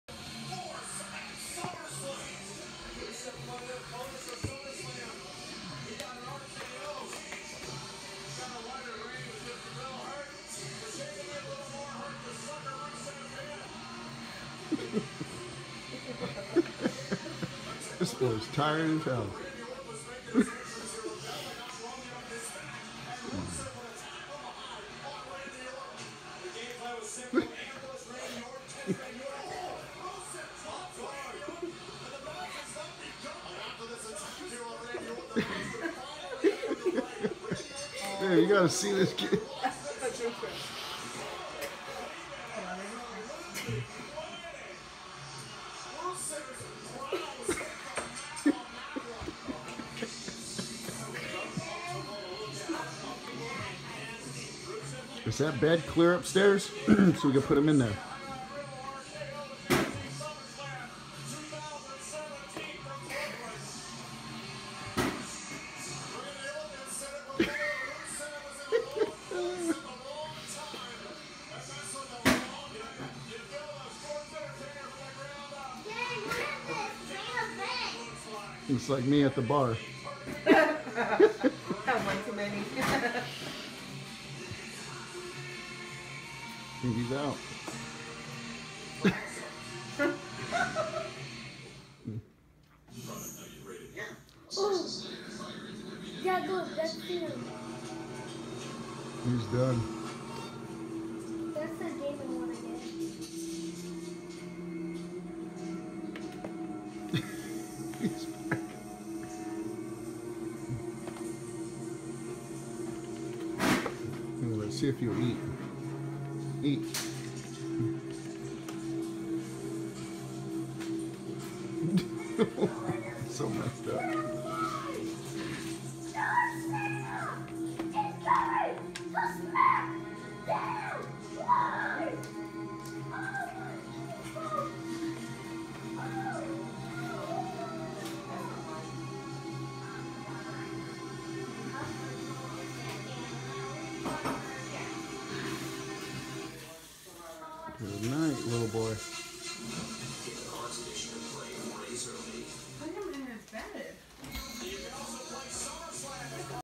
Baby falling asleep in high chair.
Baby eating and fighting his sleep. Passing out over and over!